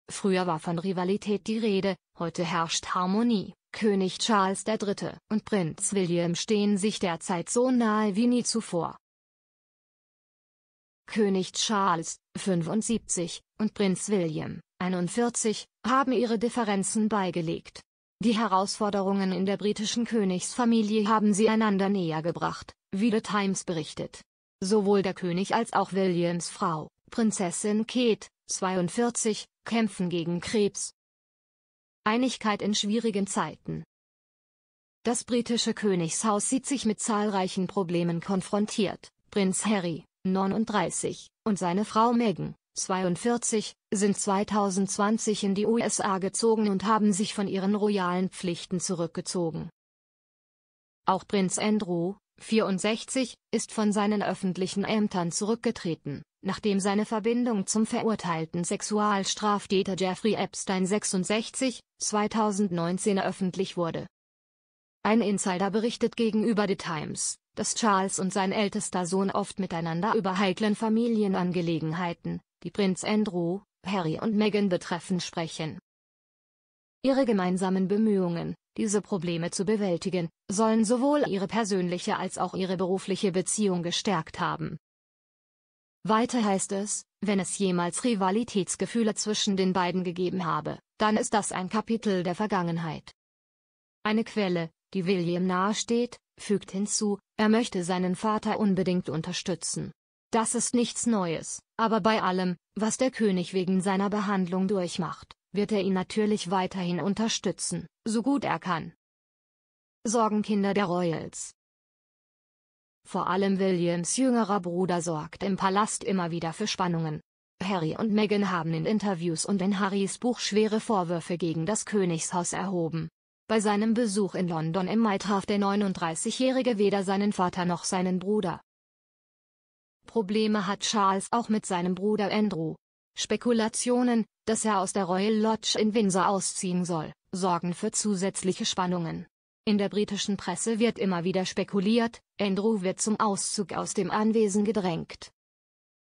König Charles & William Familienkrisen und gemeinsame Entscheidungen!
König Charles & William sind sich näher als je zuvor